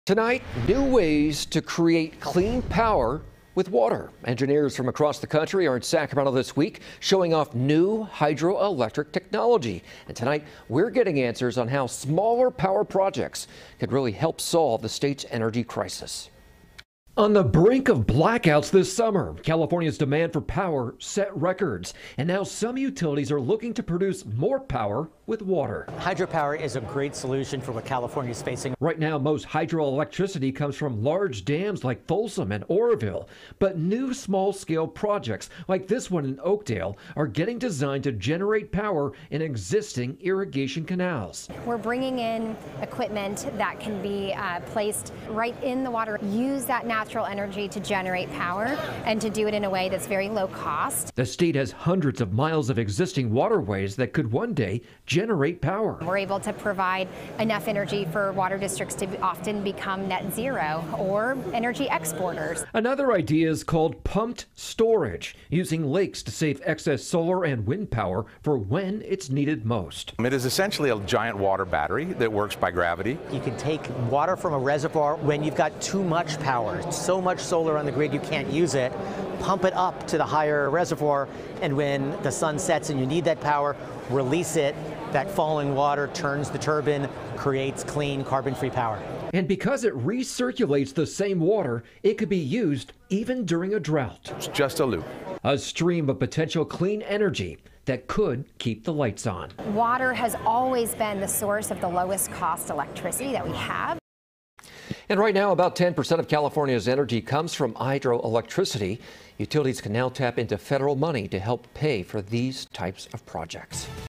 Small scale hydro-electric power plants used to solve state's energy crisis
New efforts are underway to solve the state's energy crisis with "small-scale" hydroelectric power plants. Some of the country's top hydroelectric engineers are in Sacramento this week, showing off new technology, and we're looking at how hydro could help keep the lights on.